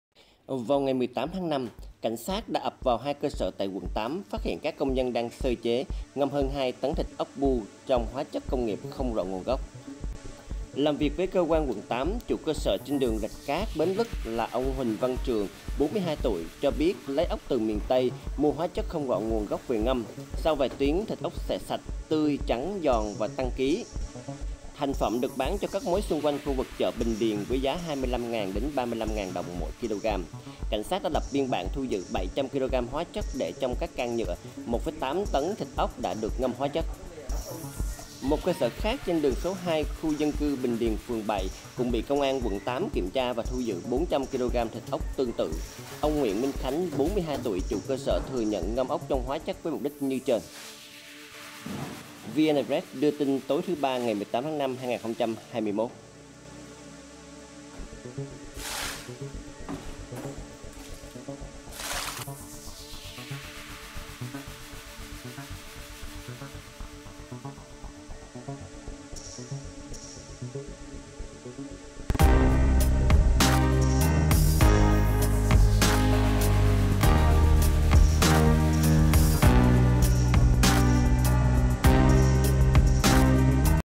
➸ BẠNCÓBIẾT ➸ 2,2 tấn ỐC ngâm hóa chất ở 📍 Quận ➑
👉 MIỆTTHỨ ❛BẠN CÓ BIẾT❜❗
2,2 tấn ốc ngâm hóa chất bị thu giữ‼️
📍 Quận ➑,

     ❝ Cảnh sát ập vào hai cơ sở tại quận ➑,
         phát hiện các công nhân đang sơ chế,
ngâm hơn ➋ tấn thịtốcbưu trong hóa chất
         công nghiệp không rõ nguồn gốc, ngày 18/5.

Làm việc với Cônganquận8, chủ cơ sở
trên đường RạchCát - BếnLức là
         Huỳnh Văn Trường, 42 tuổi, cho biết lấy
ốc từ miềnTây, mua hóa chất không rõ
nguồn gốc về ngâm. Sau vài tiếng,
         sẽ sạch, tươi, trắng, giòn và tăng ký.

         Thành phẩm được bán cho các mối xung
quanh khu vực chợBìnhĐiền với giá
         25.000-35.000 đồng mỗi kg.

         Cảnh sát đã lập biên bản thu giữ 700 kg
         hóa chất để trong các can nhựa; 1,8 tấn
         thịt ốc đã được ngâm hóa chất...

         Một cơ sở khác trên đường số 2 Khu dân cư
         Bình Điền, phường ➐, cũng bị Công an quận ➑
         kiểm tra, thu giữ 400 kg thịt ốc tương tự.

         Ông Nguyễn Minh Khánh (41 tuổi, chủ cơ sở)
         thừa nhận ngâm ốc trong hoá chất với mục đích
         như trên.❞

        📽️ Công an cung cấp
📸 Nhật Vy @